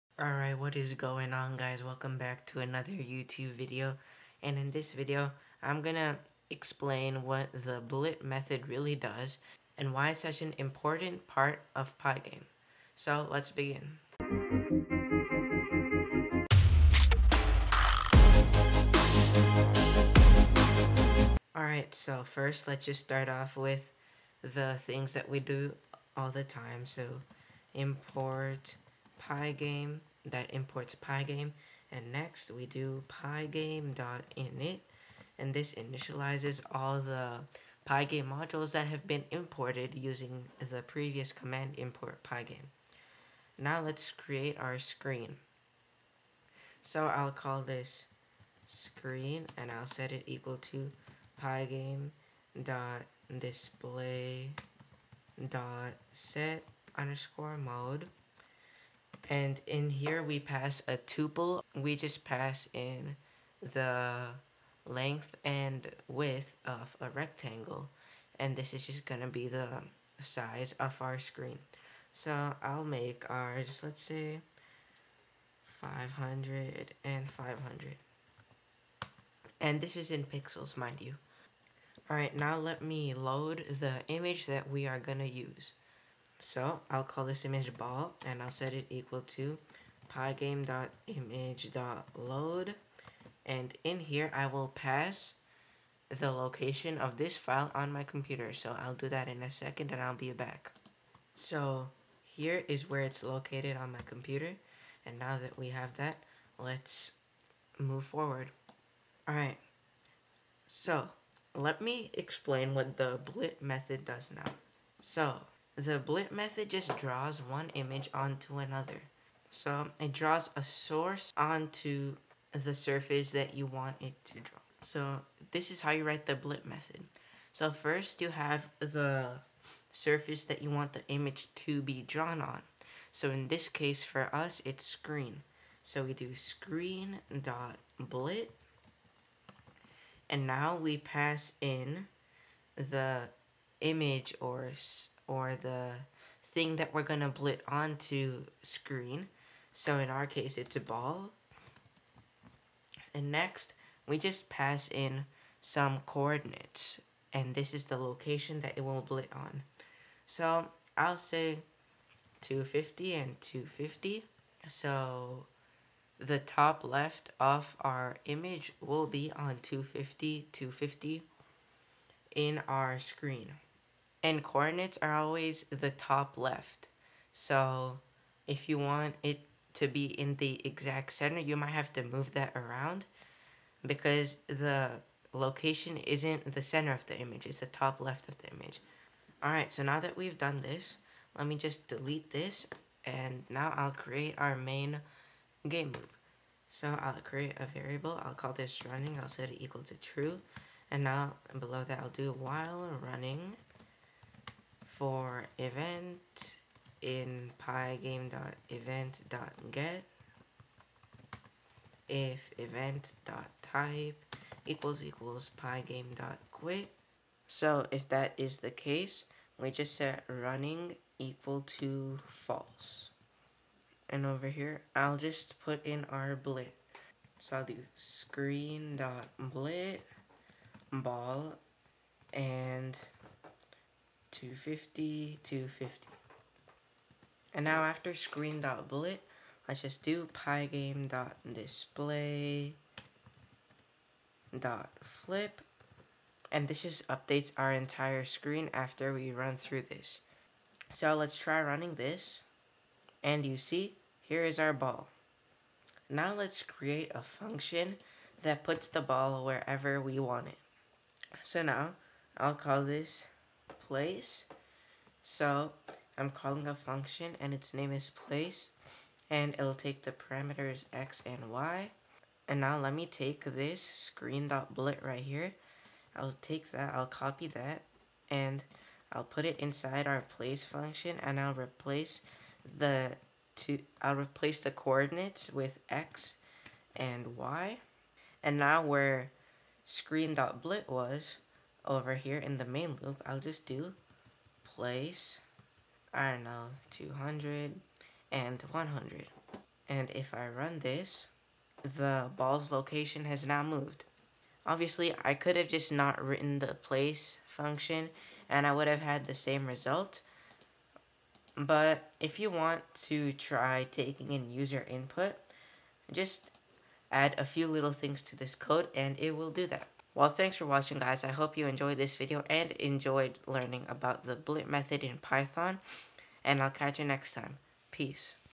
Pygame.surface.blit EXPLAINED!!
In this video I explain pygame.surface.blit and why it is so important.